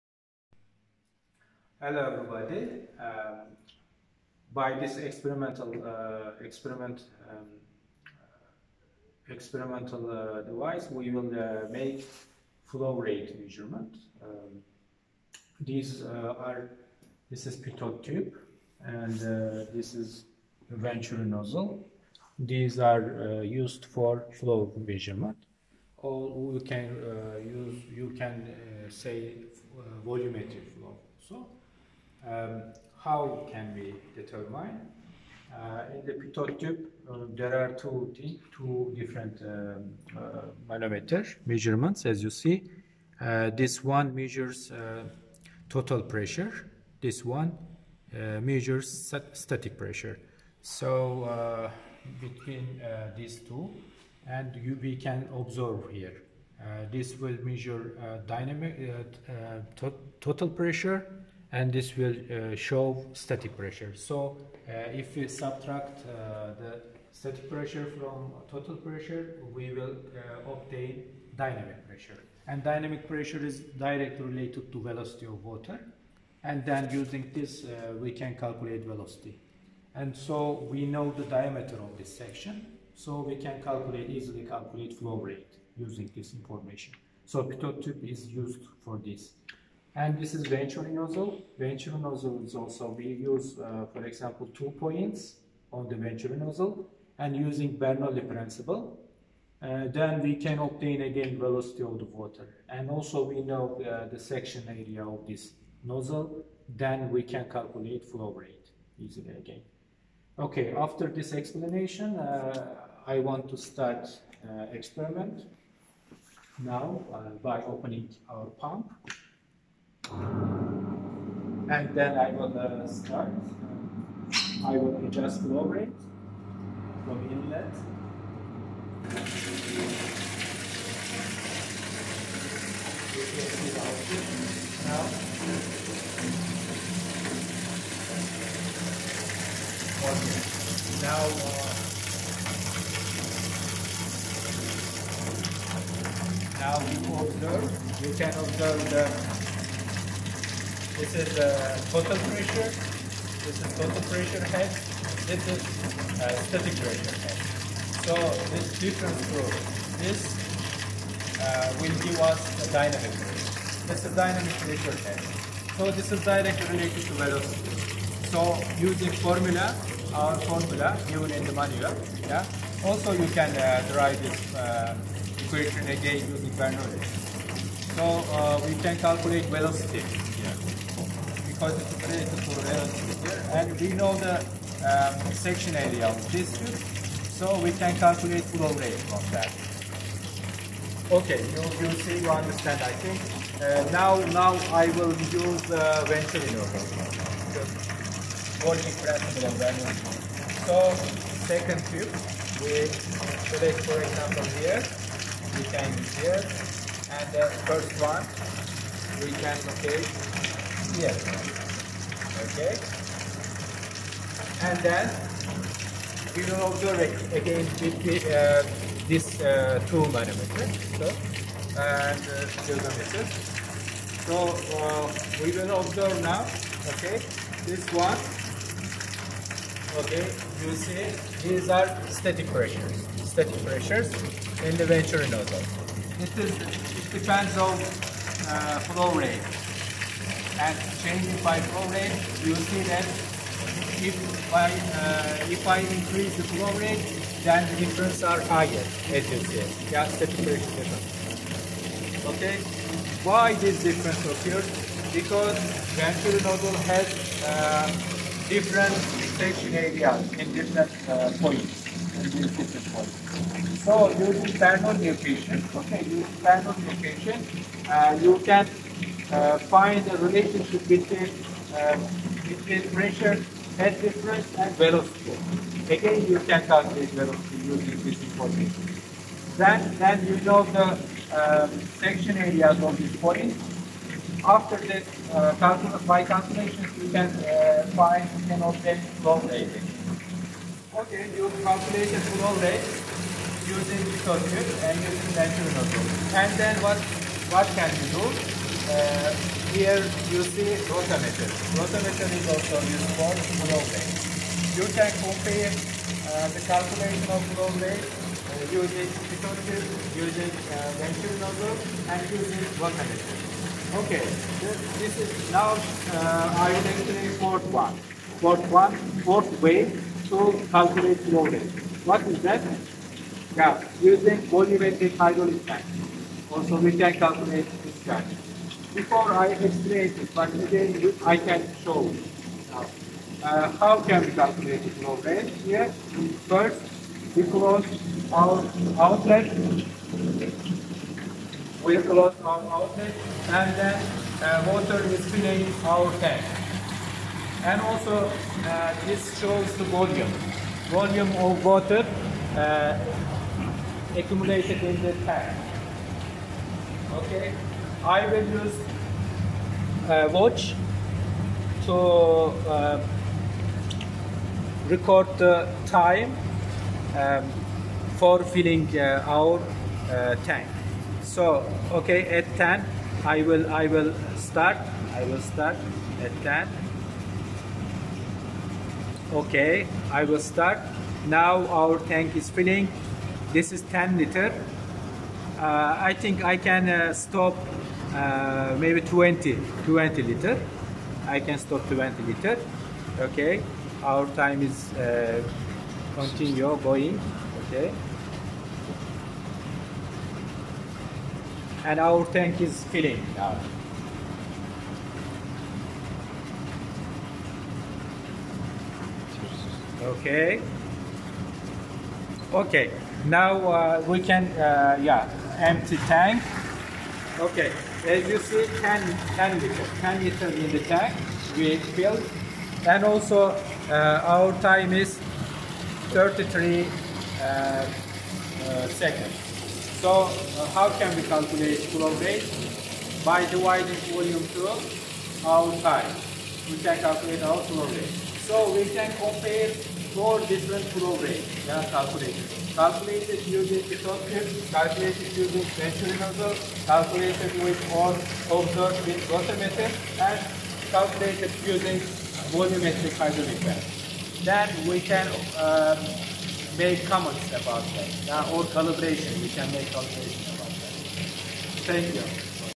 Flow Measurement Experiment | Pitot Tube, Venturi Nozzle & Hydraulic Bench
This video demonstrates an experiment on flow measurement using a Pitot tube, Venturi nozzle, and hydraulic bench. We explore how these devices are used to measure fluid velocity and flow rate in hydraulic systems. The experiment highlights key principles of fluid dynamics and practical applications in engineering. I hope you find this tutorial helpful! This video was prepared in Ilia State University. I would like to thanks Irakli Salukvadze who helped me in recording this video.